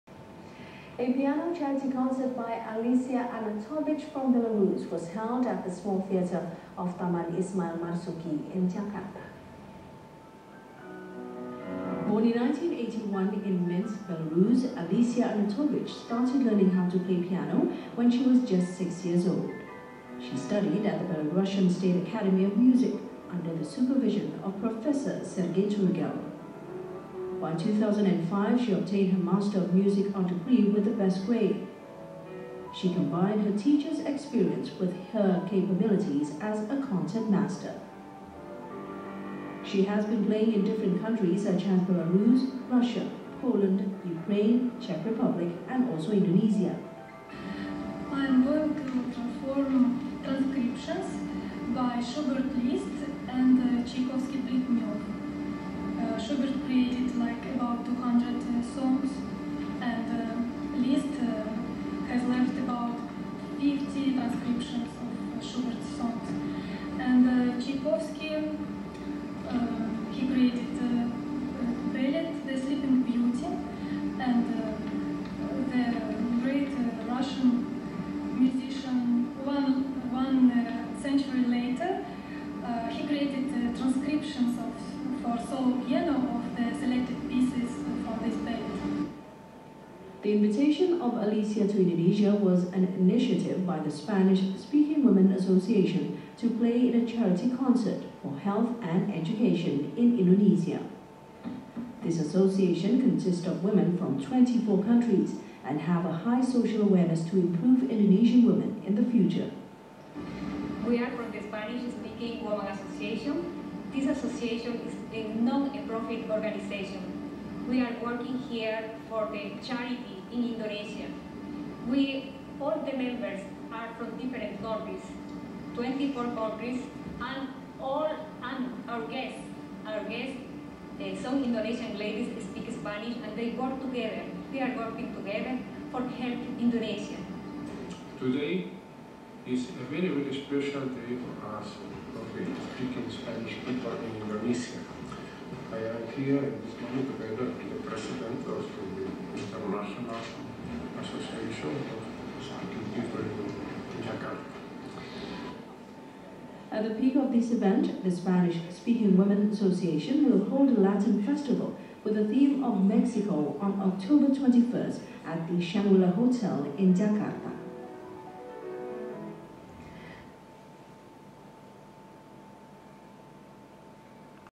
Alesia Arnatovich, concert in Jakarta, Indonesia (29.May.2011)
Indonesian TV information about "Transcritpions!", solo piano concert performed by the belarusian artist Alesia Arnatovich the past May 29th 2011. The performance was in Taman Ismail Marzuki (Teater Kecil), Jakarta, Indonesia.